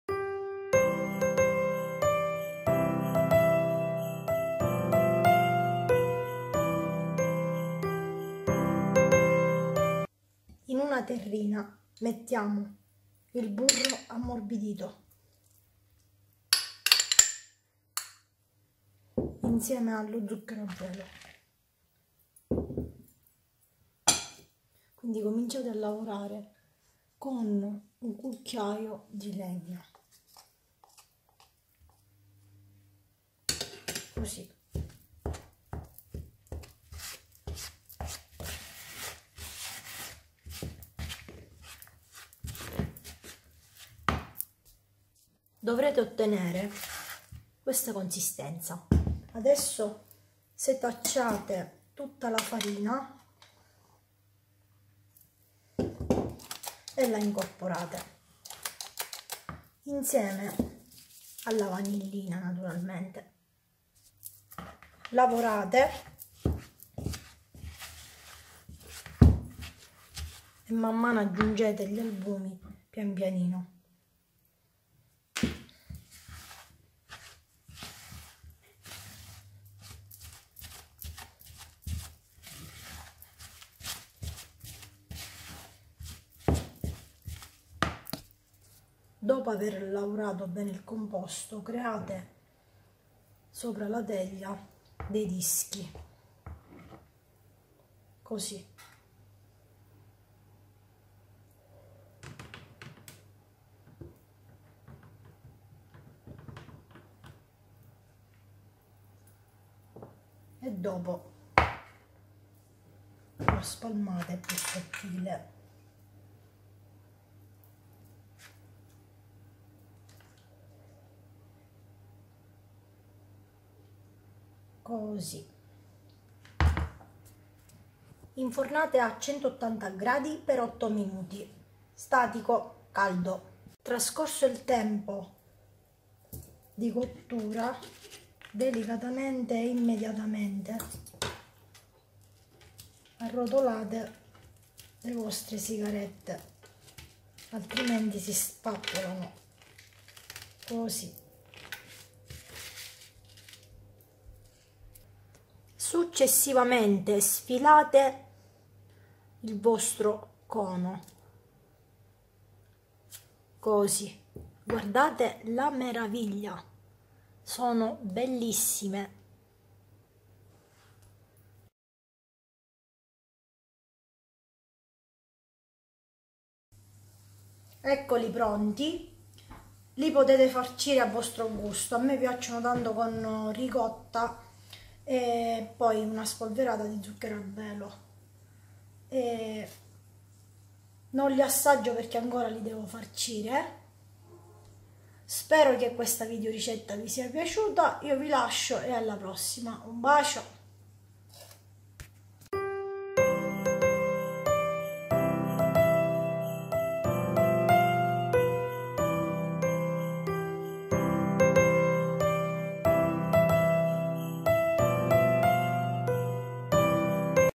SIGARETTE DOLCI
Oggi vi suggerisco i biscotti sigarette dolci, da farcire a propio gusto, sono buonissime con la ricotta. Da preparare con e senza bimby.